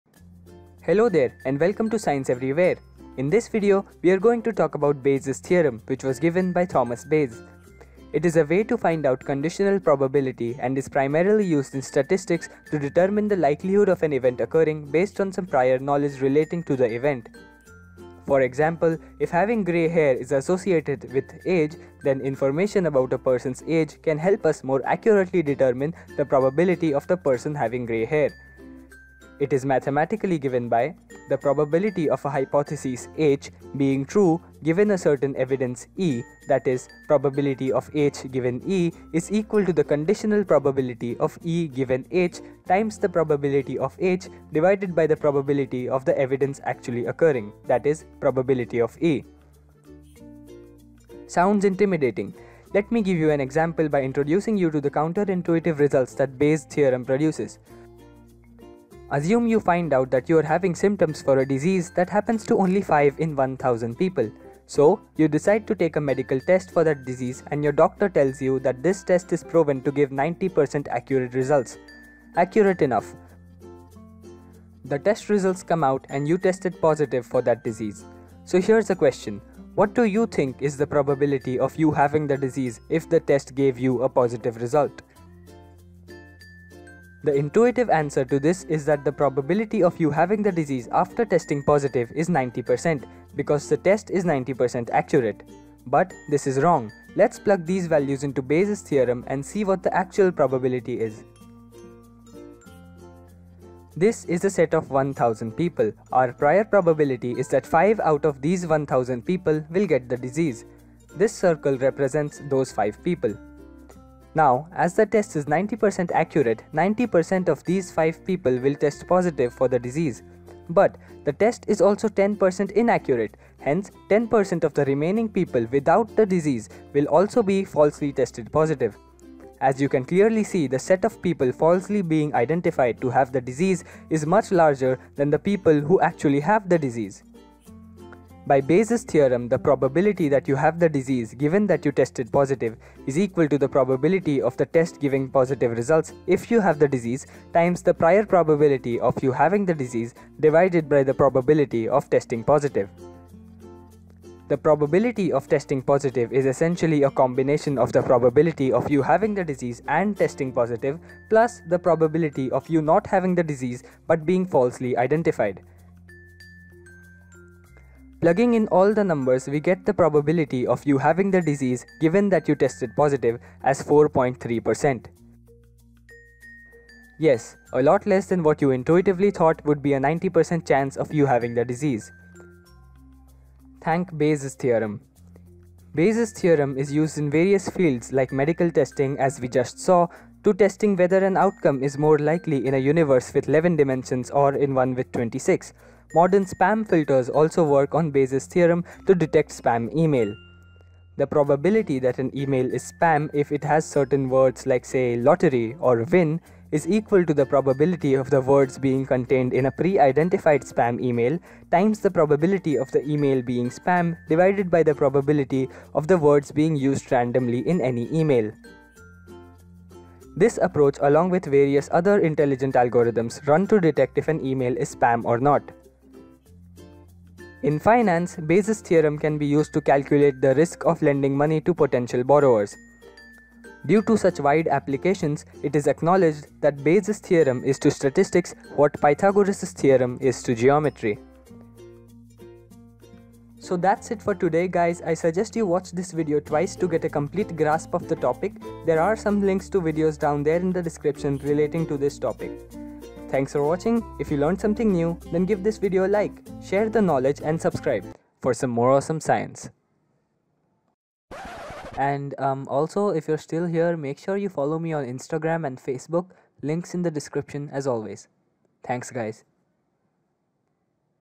Bayes' theorem and why medical test results are misinterpreted
Bayes' Theorem is widely used in statistics as well as computer science, data analytics etc. So I just made a video because this theorem is awesome.
The point of this video is not to make you understand the math but the misinterpretation of medical  test results. I also highlight some other uses of Bayes' theorem. Enjoy guys!

Creator: Yash Chavan